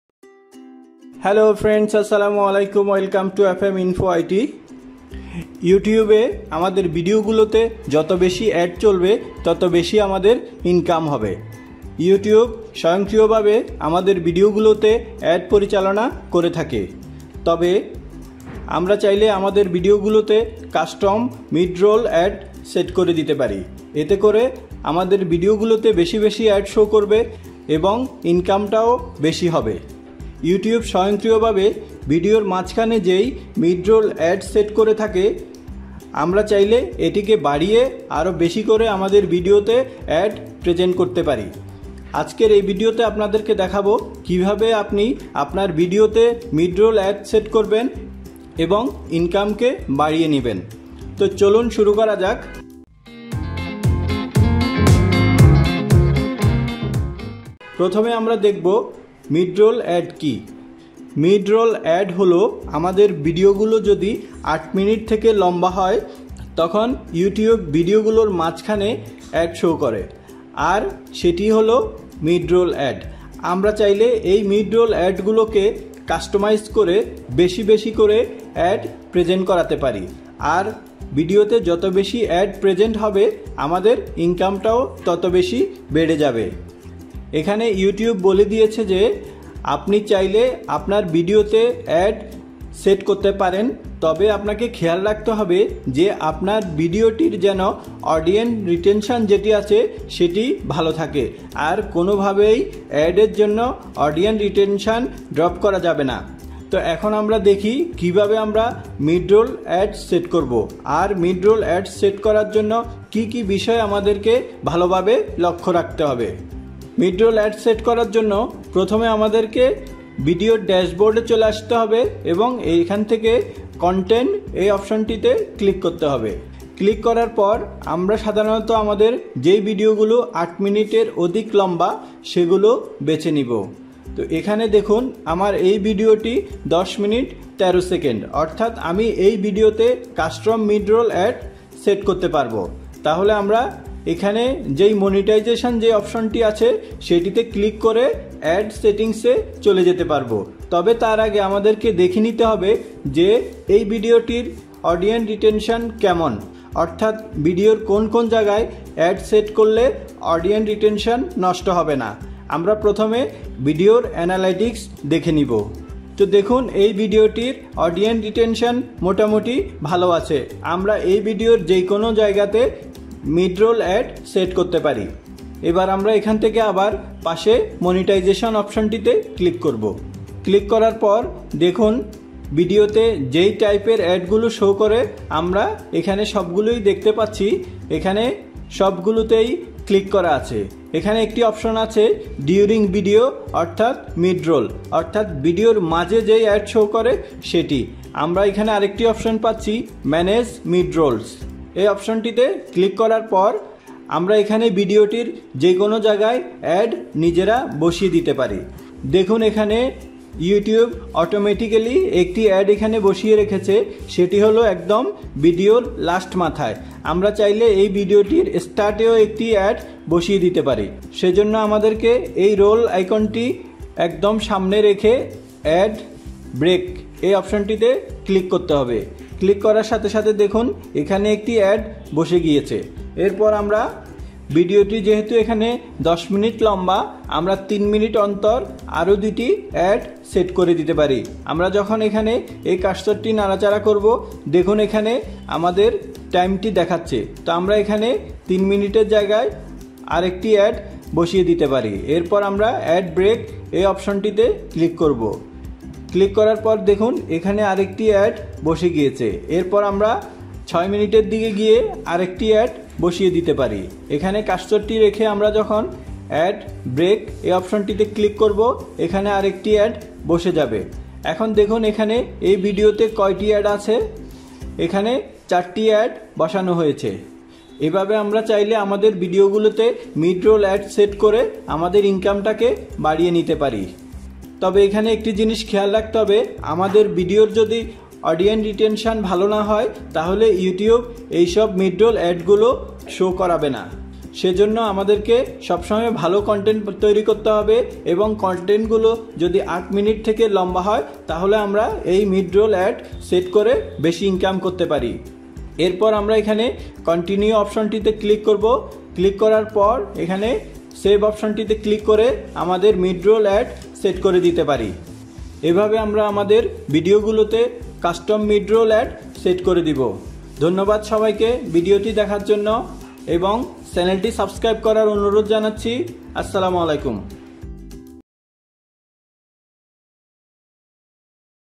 How to Set mid roll ads on Youtube video | Increase Your Youtube Revenue | FMInfo IT
How To Set Mid Roll Ads On YouTube Video | Increase Youtube Revenue - This video showing how to set mid roll ads on your youtube video and how to increase youtube Revenue.

ইউটিউবে আমাদের ভিডিও গুলোতে স্বয়ংক্রিয়ভাবে Ads শো হয়, আর এই Ads এর পরিমাণ যত বেশি ইনকামও হবে তত বেশি। আমরা চাইলে আমাদের ভিডিও গুলোতে Mid-Roll-Ads সেট করে বেশি অ্যাড শো করাতে পারি।
এই বিষয় বিস্তারিত এই ভিডিওতে আলোচনা করা হয়েছে। আশা করি ভিডিওটি ভালো লাগবে। যদি ভিডিওটি আপনাদের সামান্যতম উপকারে আসে তাহলে অবশ্যই লাইক👍, কমেন্ট 📰এবং শেয়ার📤 করার অনুরোধ জানাচ্ছি।

🌹ধন্যবাদ🌹